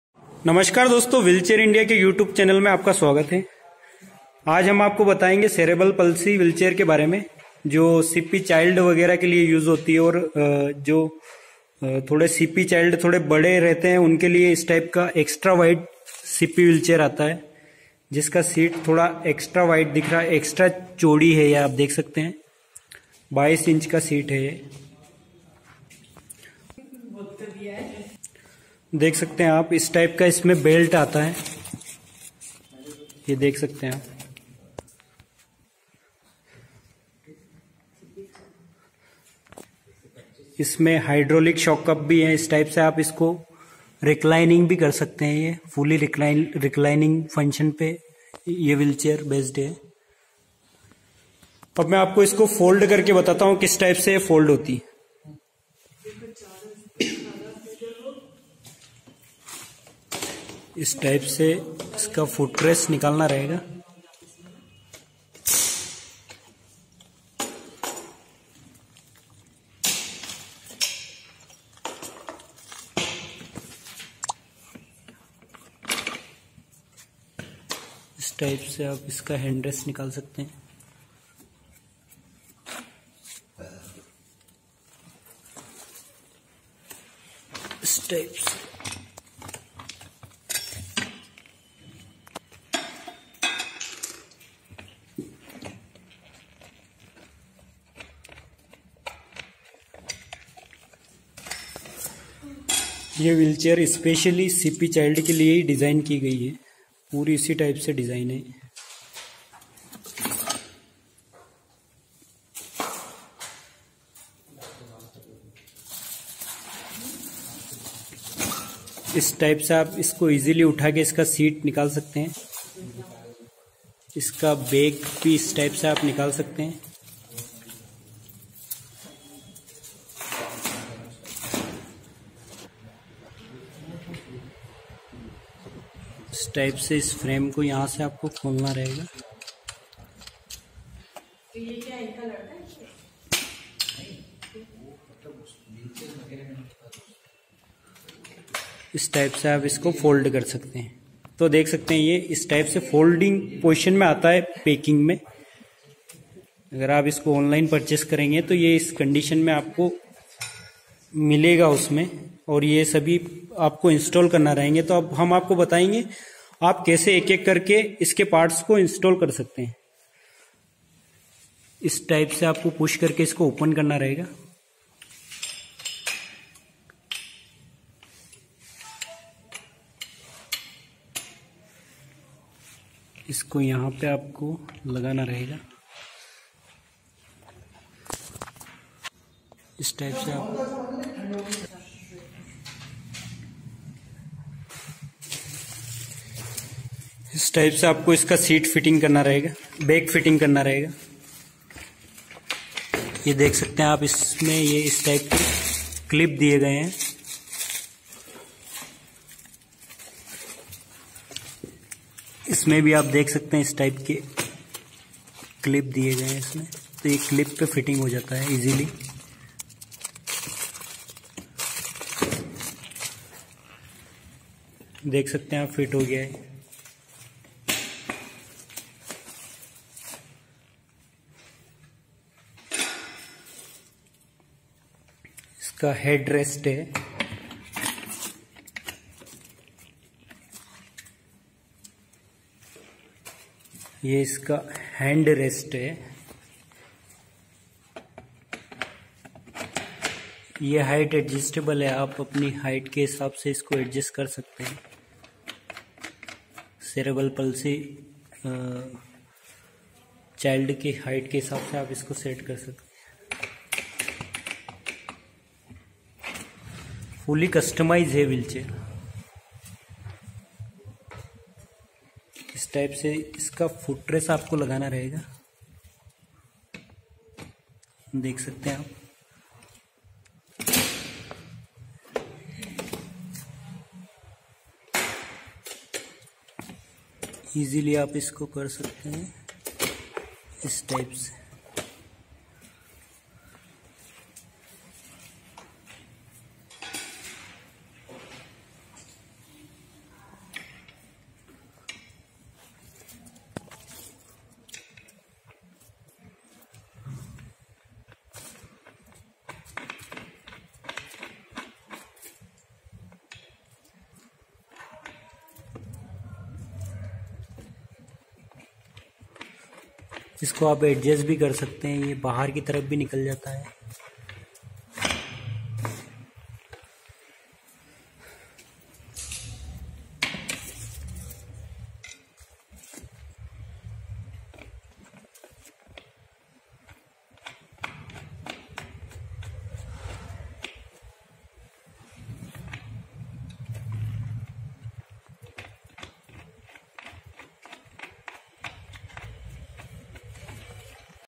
Multi Functional Cerebral Palsy Recliner Wheelchair For Adult Extra Wide CP Wheelchair
Cerebral Palsy CP Pediatric Multifunction Wheelchair 18 Inch Seat
Features

The modular design offers quick and convenient assembly, folding and disassembly
The multi-functional headrest can adjust to the most desirable angles.
Frame weights only 11kg once folded; nimble and easy to transport
Suitable for: medium to serious CP children
Cerebral Palsy CP Pediatric Multifunction Wheelchair

The design and development of a personal CP wheelchair is quite a challenge for every wheelchair manufacturer.
The all-new Cerebral Palsy CP Pediatric Multifunction Wheelchair is the proud result of three years of collective efforts between Karma and senior German medical engineers.
Continuing the great traditions of its predecessors, the 3rd generation Karma CP chair boasts many revolutionary design features that are second to none.
Fixed seat and backrest system; ready for later upgrade
Tilt-in-space seat offers pressure relief and a shift in the center of gravity
Compact when folded, requiring minimal storage space
The model designed for cerebral palsy children only.
Ultra-lightweight aluminum alloy frame
Epoxy powder coated frame
Detachable armrest & footrest provided
Elevated and swinging footrest
Elevated footrest provided to elevate leg angle
Height adjustable and detachable headrest
Hydraulic reclining high back for a comfortable posture
Hydraulic adjustable seat angle
Detachable back and seat pad
Extra cushion upholstery provided to underarm, head & calg
Foldable
Lever and paddle brakes provided
Safety belt provided
Maintenance-free rear solid wheels
Cloth look like waterproof upholstery
Anti wheels for better safety and stability
Extra cushion upholstery provided to underarm, head & leg
Folding action